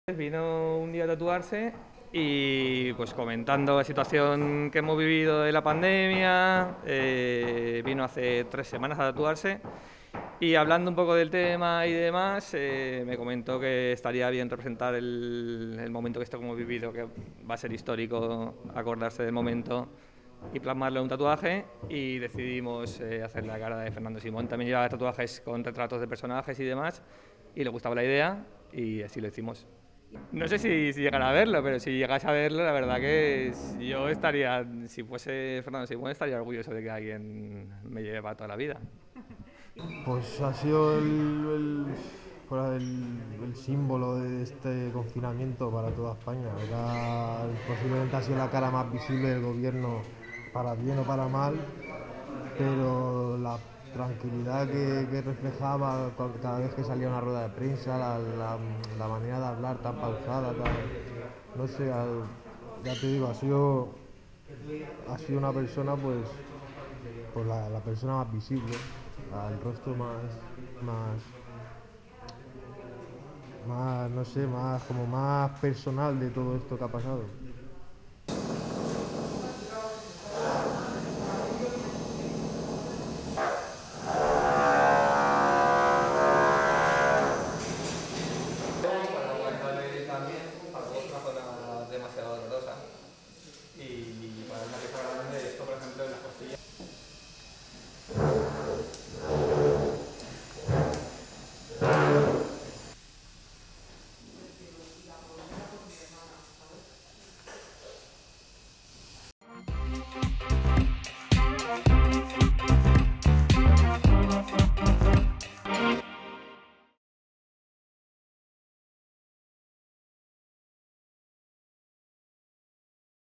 El tatuaje más viral de la pandemia
Un joven valenciano ha decidido hacerse un tatuaje con rostro del director del Centro de Coordinación de Alertas y Emergencias, Fernando Simón, en su pierna por ser "el símbolo del confinamiento de toda España" y "la cara visible de la situación que hemos vivido". El artista Nacho Frías del estudio Noble Art es el creador de este tatuaje, quien ha señalado que si fuera Simón se sentiría "orgulloso" el hecho de que "alguien me quisiera llevar toda la vida".
Fuente: Europa Press